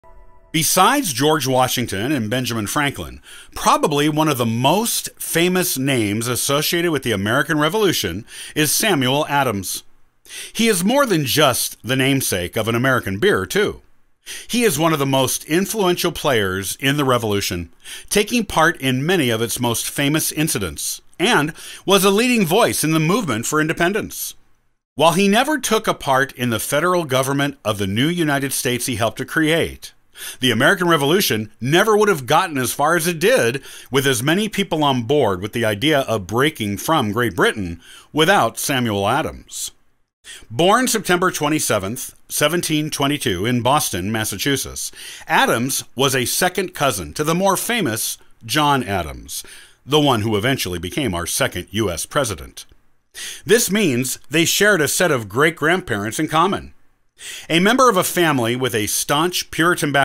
Have you heard of SAMUEL ADAMS? Not the popular American beer.
Have you ever wondered who Samuel Adams was beyond the popular American beer brand that bears his name and face? He is actually one of the most notable figures from the American Revolution. Read on to discover the true story of this remarkable patriot.